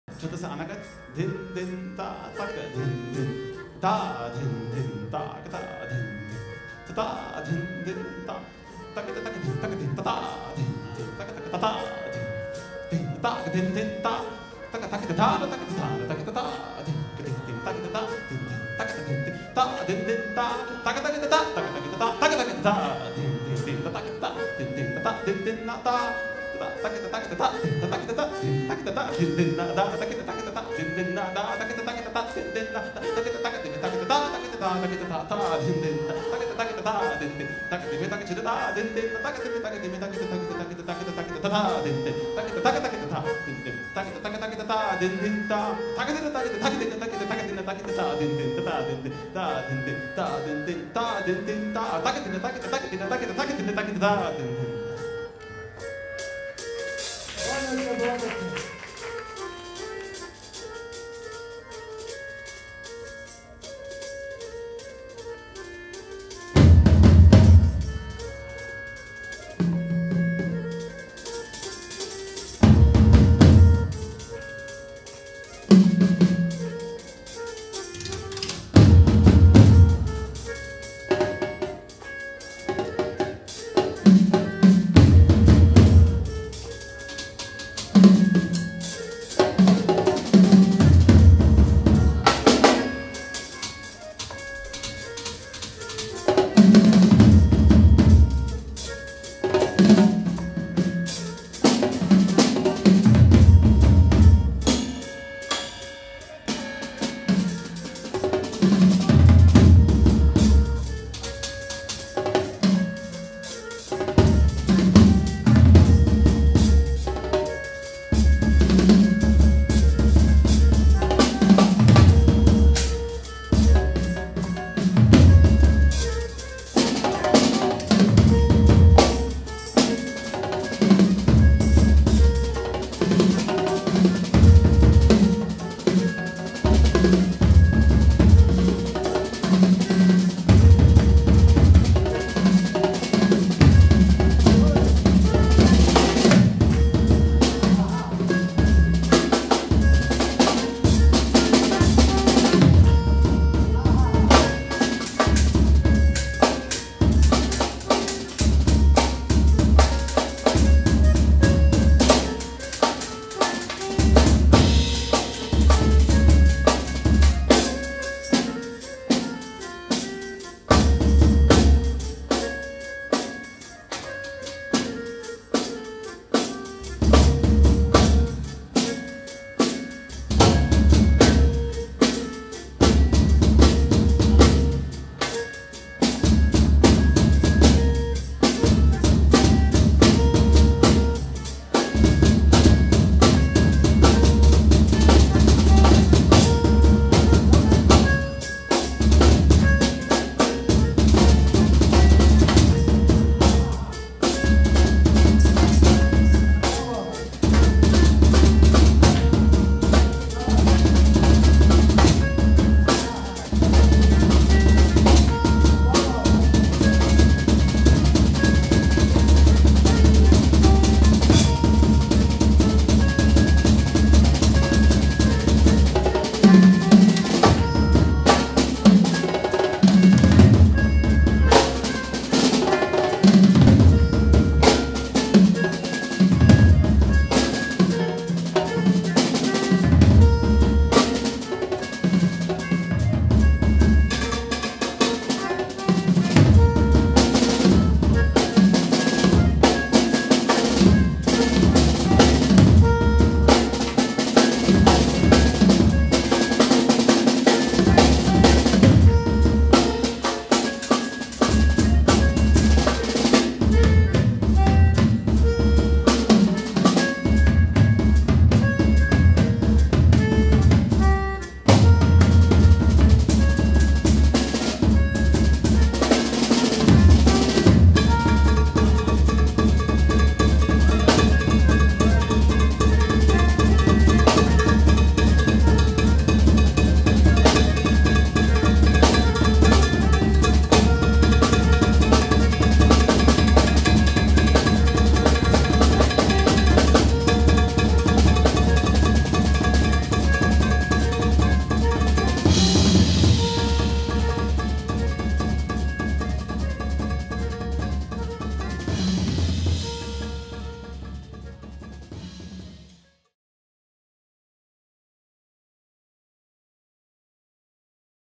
Guru Purnima Drumsolo ・Anagath Teentaal ・Bernhard Schimpelsberger
Bernhard Schimpelsberger ・ The Guru Purnima Concert

Performed at Taal Yogi Pandit Suresh Talwalkars Guru Utsav festival Pune / India
28/8/2015

Bernhard Schimpelsberger - drums
Milind Kulkarni - harmonium

watch HEART OF RHYTHM - a short docu about Bernhards return to India and the making of this concert here: